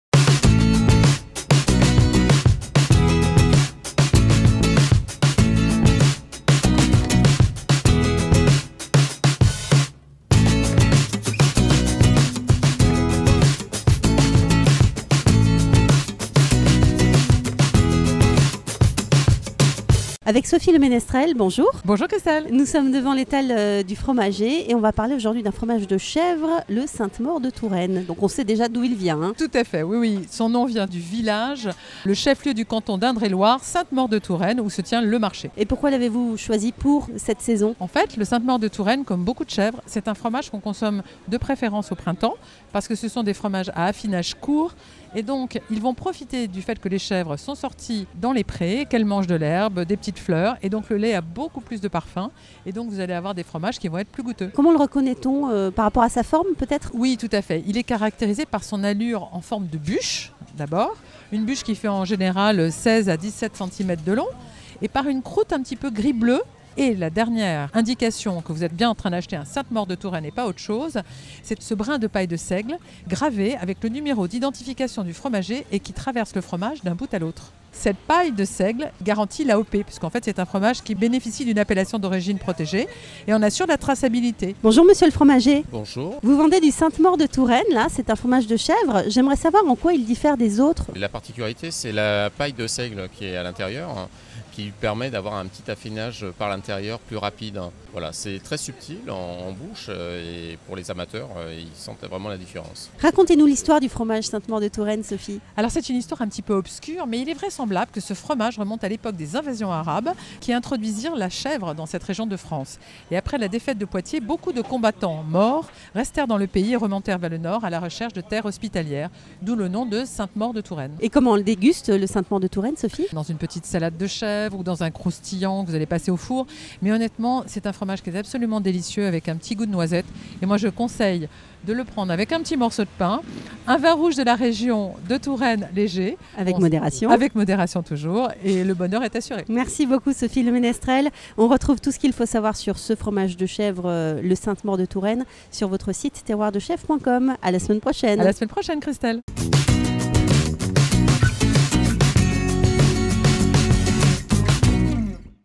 Terroirs de Chefs - "C'est de saison : le Sainte-Maure de Touraine ! "
"C'est de saison": la chronique de Sophie le Menestrel.

Aujourd'hui, Sophie le Menestrel nous parle du Sainte-Maure de Touraine.
Chaque semaine un produit de saison à découvrir et à cuisiner avec les recettes de en partenariat avec Radio VINCI Autoroutes